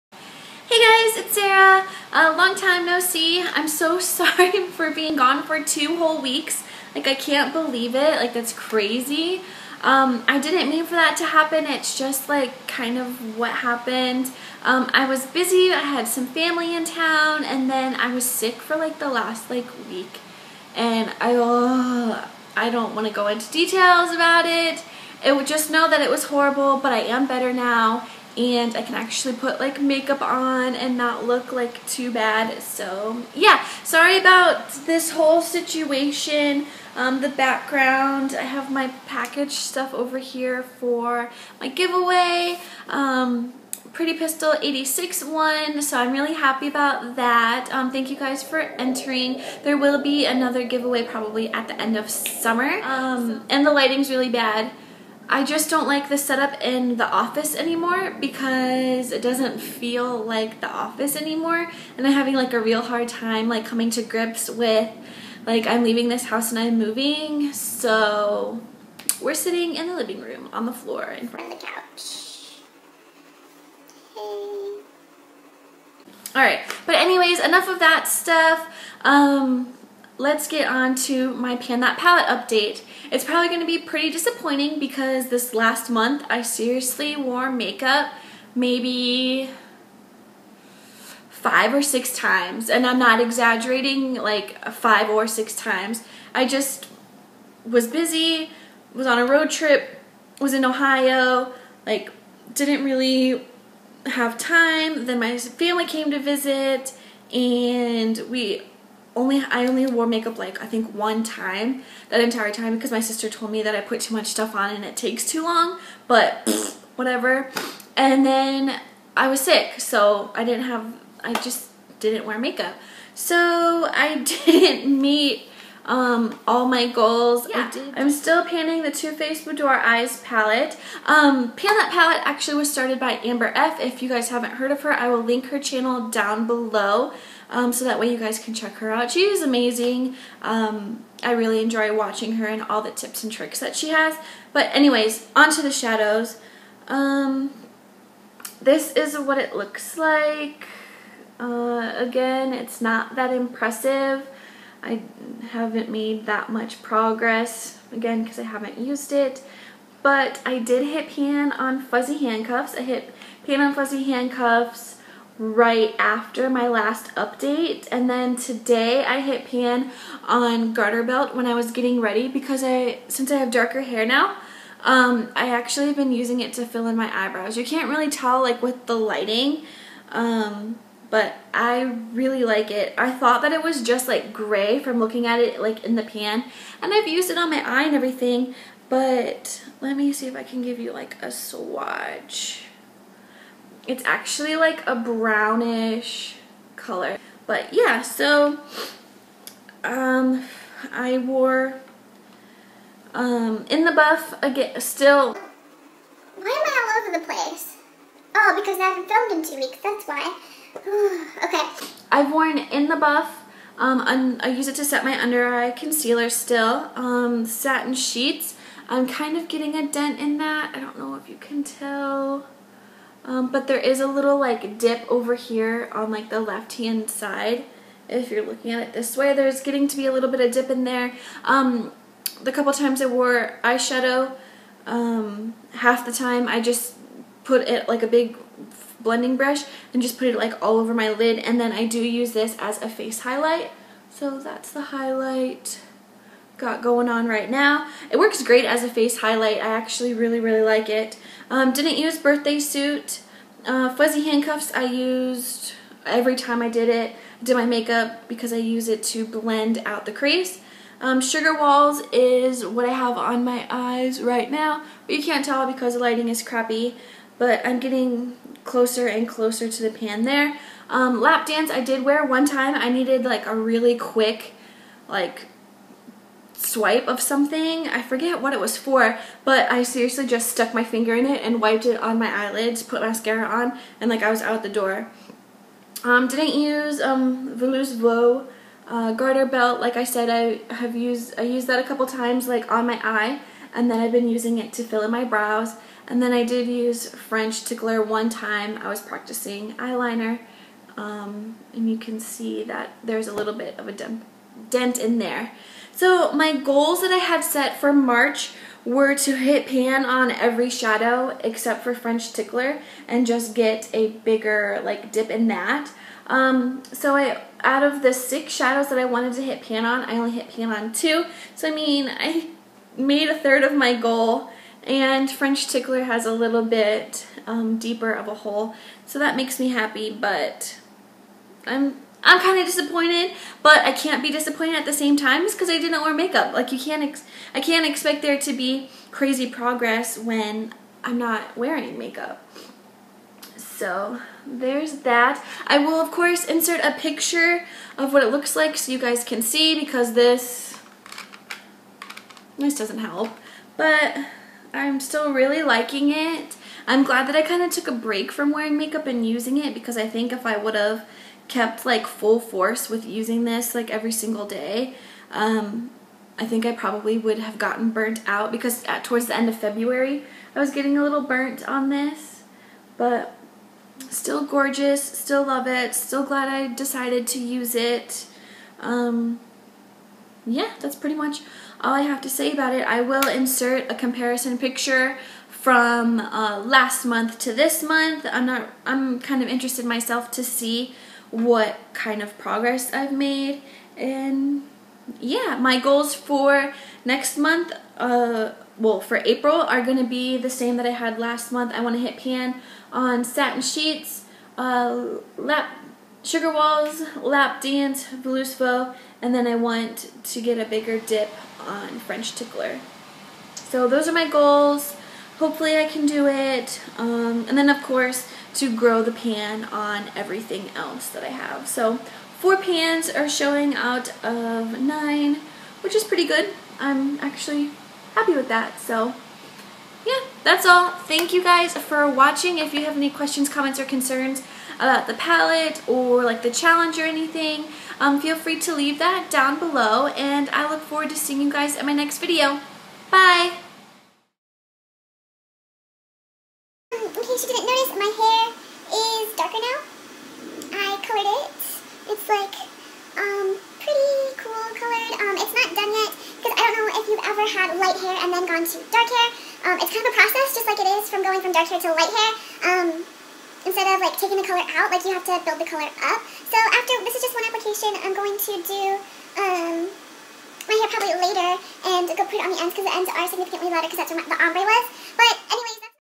Pan That Palette: Update # 3
LOOOOOOOOOOOOOONG TIME NO SEEE!

So sorry to have been gone for TWO WEEKS! Anywhooo, here is the update for my Pan That Palette Challenge on the Too Faced Boudoir Eyes palette!!